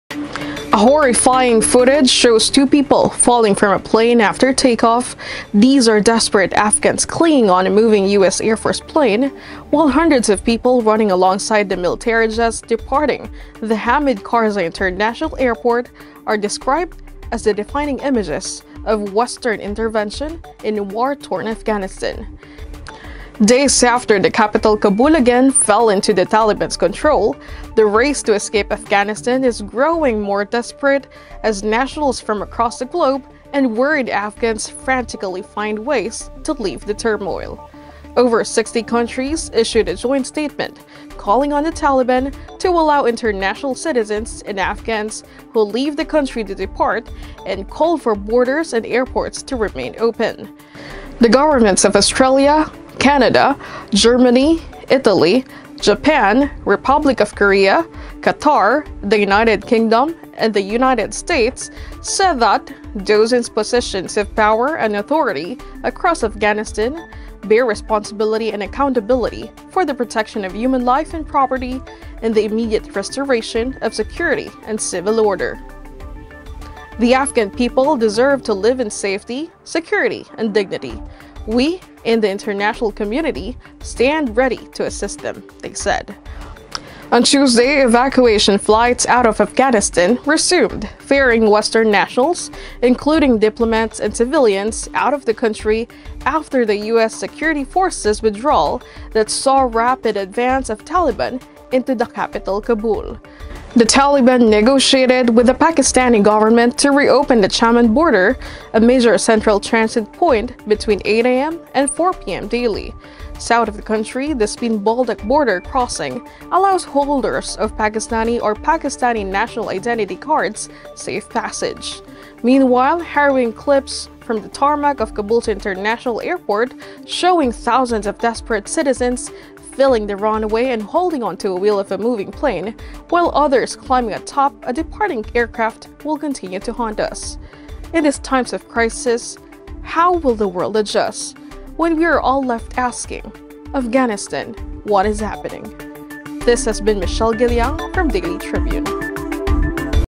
The race to leave Afghanistan
Who can leave?

Horrifying footage of two people falling from a plane after takeoff, desperate Afghans were clinging on a moving U.S. Air Force plane, hundreds of people running alongside military jets departing the Hamid Karzai International Airport are described as the "defining images" of Western intervention in war-torn Afghanistan.